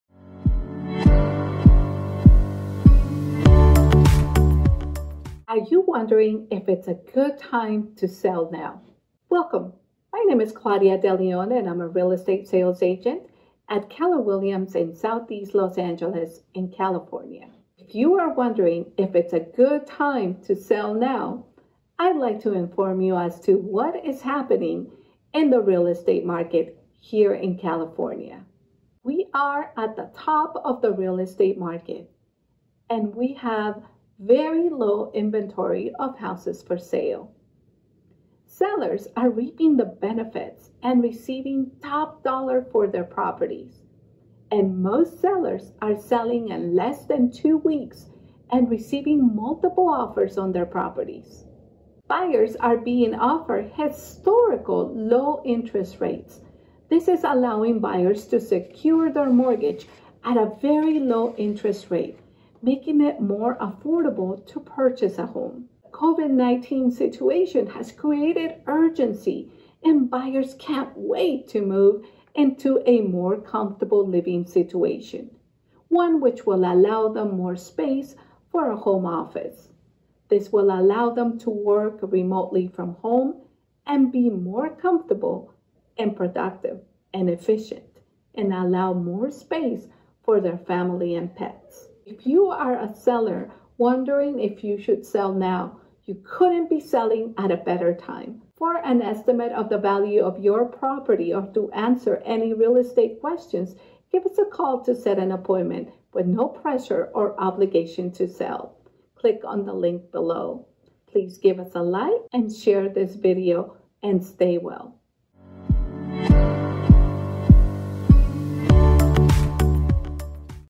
Is it a good time to sell a house
This is an informative video about the current real estate market in California. It describes what is going on in the real estate market during COVID-19 and it is impacting sellers and buyers in the marketplace.
If you have additional questions or suggestions for future videos please leave suggestions in the comments. Thank you!